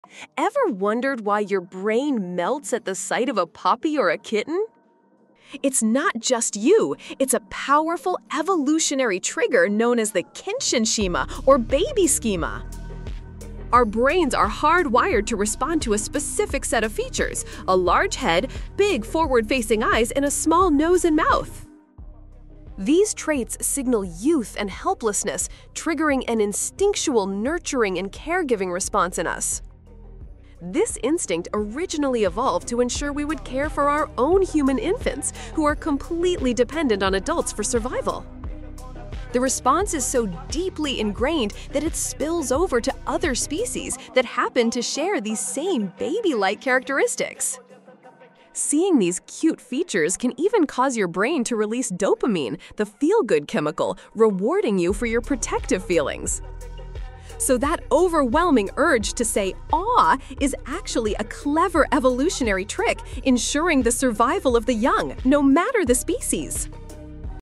The Evolutionary Trick That Makes You Love Baby Animals
Discover the science behind why we find baby animals so irresistibly cute. It's an evolutionary instinct called the 'baby schema' that's hardwired into our brains to ensure survival.